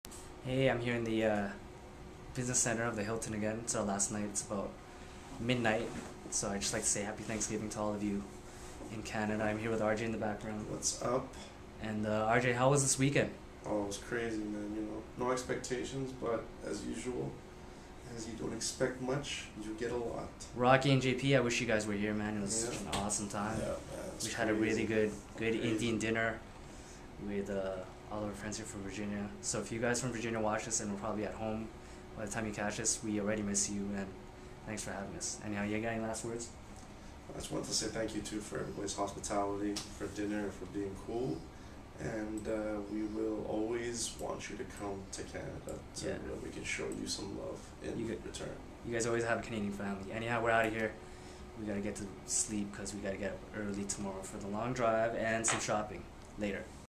blog: last day in the V-A
10.11.09
on the road in the morning, homebound. but first, we dine at a fine indian restaurant with our american family and the thornhills after a long, productive day in d.c.